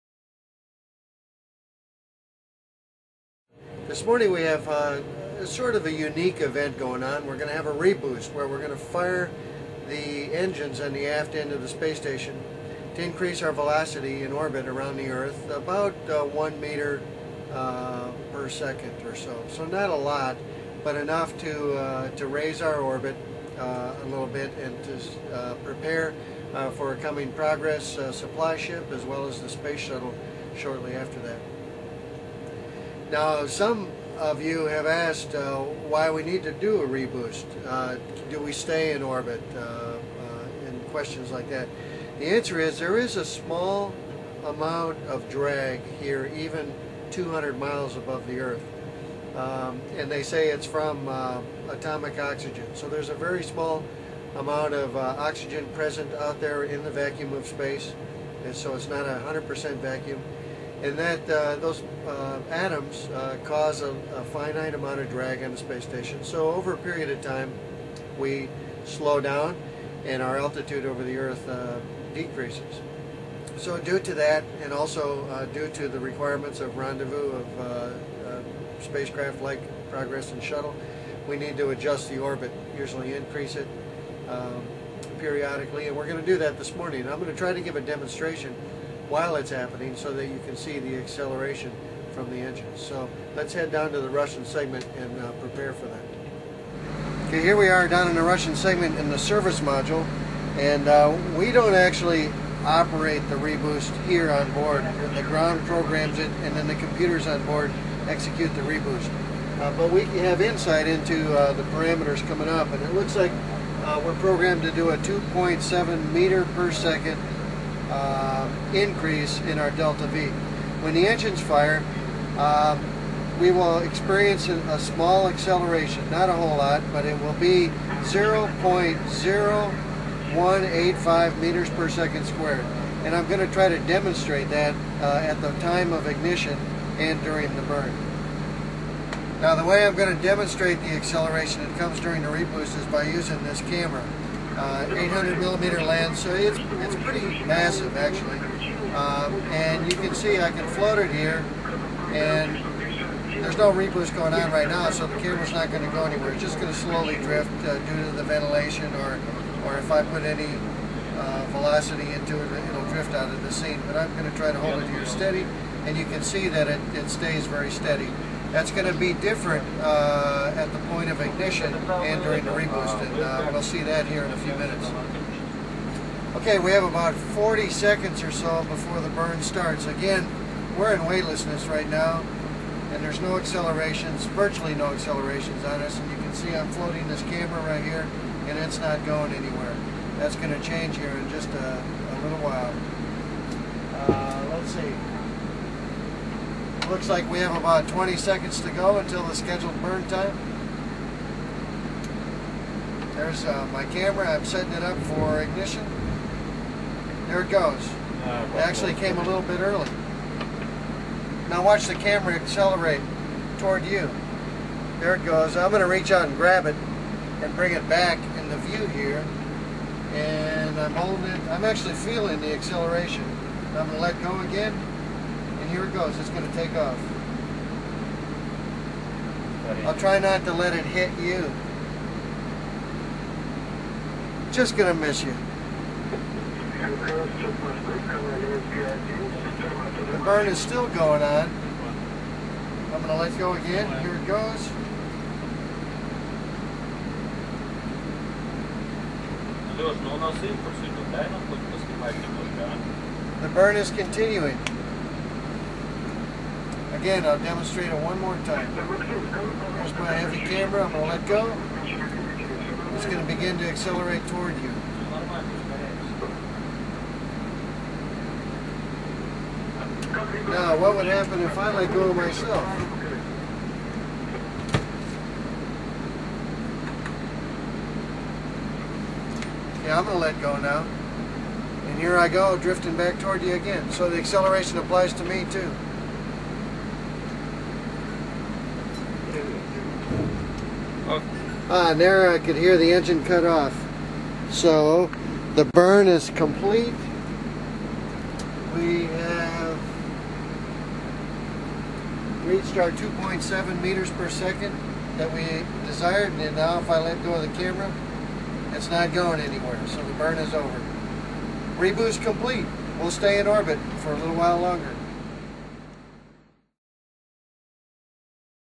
🔴 Space Station Reboost
Astronaut Jeff Williams demonstrates the acceleration experienced inside the cabin during a planned International Space Station reboost on January 24, 2010. The ISS is reboosted periodically to maintain its orbit, and to prepare for visiting spacecraft, such as the space shuttle and Progress vehicles.